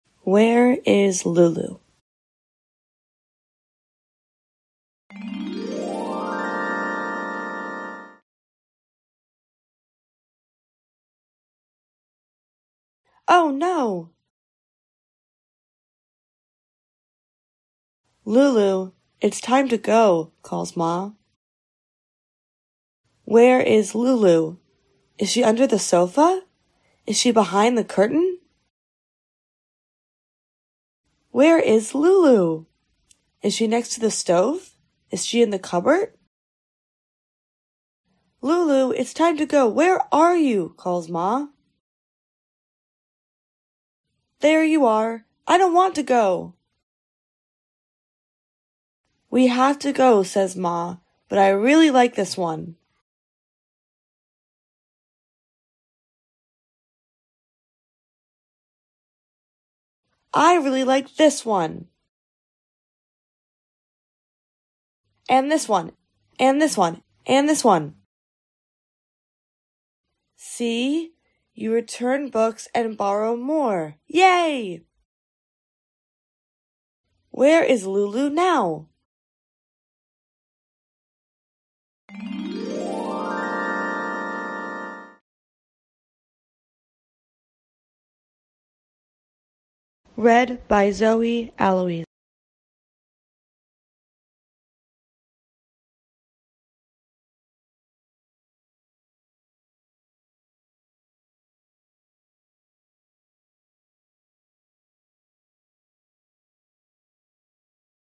[1.3] - Where is Lulu? - (read aloud video story book @ Anglihel.com)
Where is Lulu?
by
Clyde Beech (Illustrator)
Lisa Treffry-Goatley (Editor)
Mohale Mashigo (Writer)
Nkosingiphile Mazibuko (Designer)

Where is Lulu and why is she hiding?

Readability Level = 1.3
Read by Zoe Aloise

"Where is Lulu?" by by Mohale Mashigo, Nkosingiphile Mazibuko and Clyde Beech

and is available on this YouTube channel or at Anglihel.com